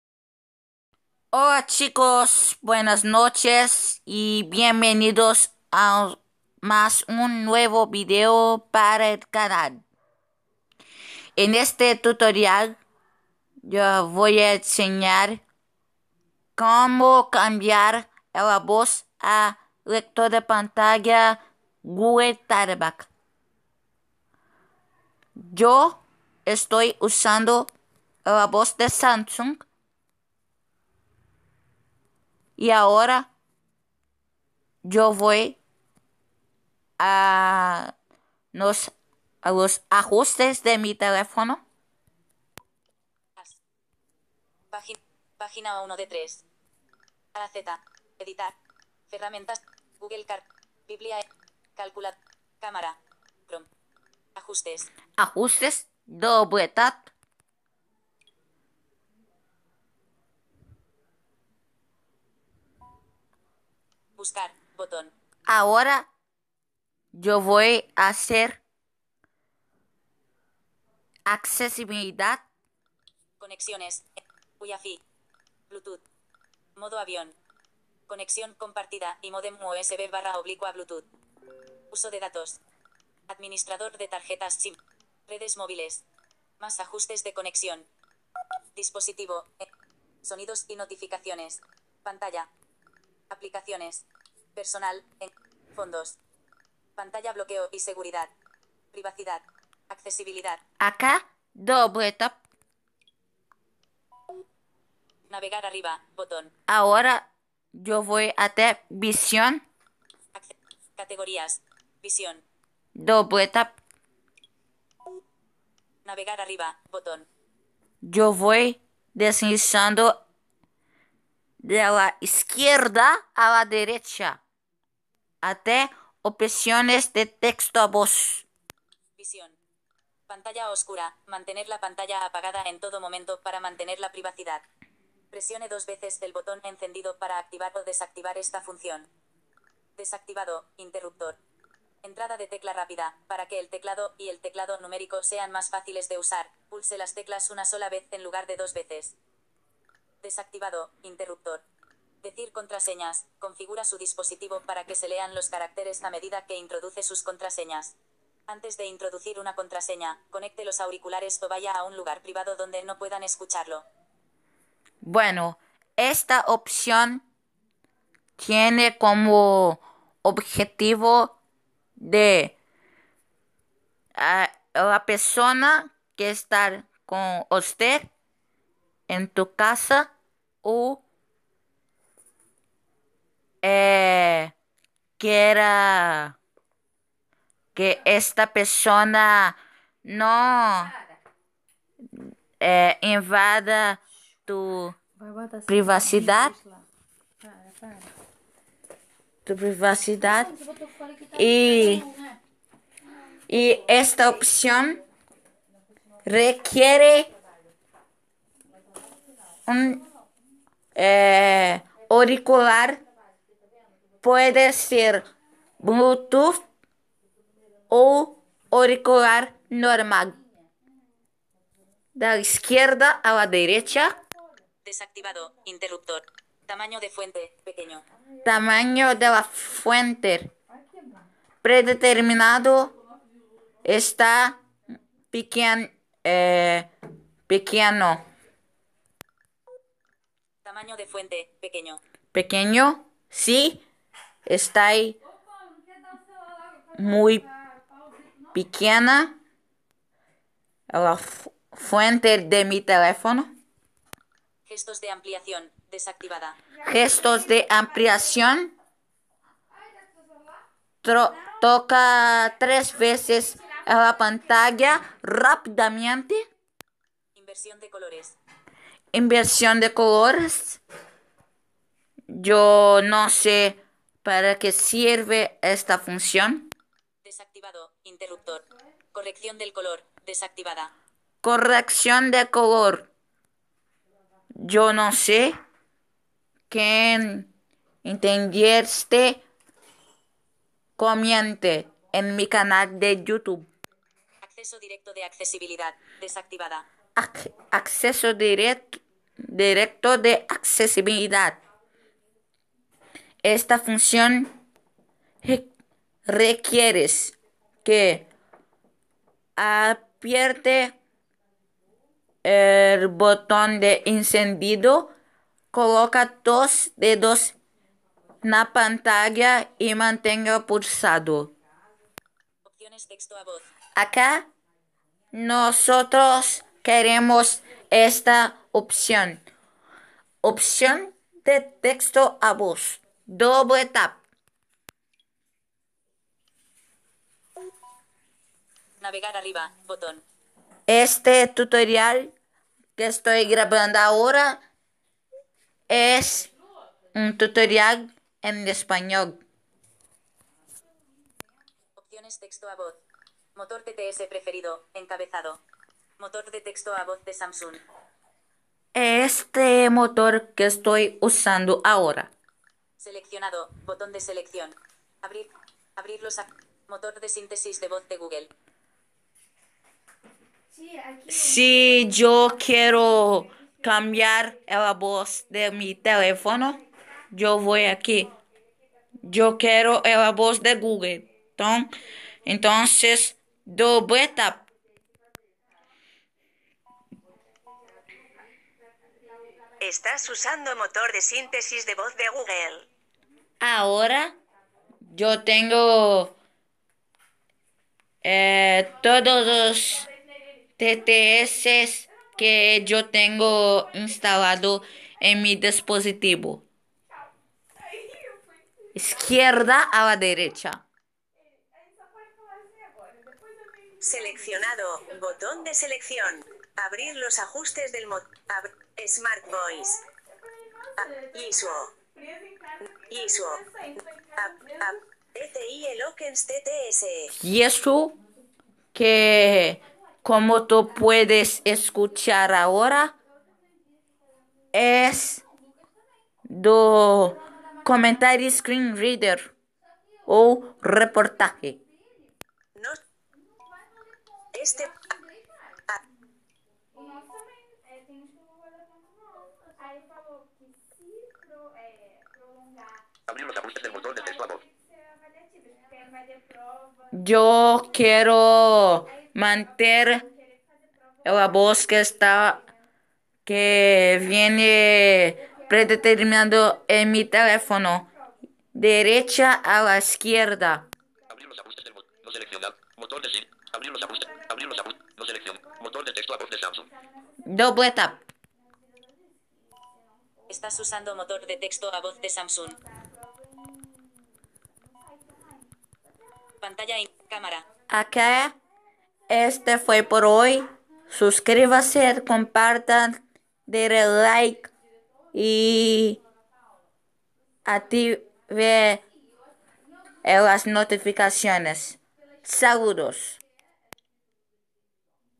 Tutorial en español
Hola tchicos, bien venidos a uno nuevo video para el canal. Esta ocasion yo voy assenhar como cambiar ela vos a google talkback. Gracias por 46 sucritotes en mi canal.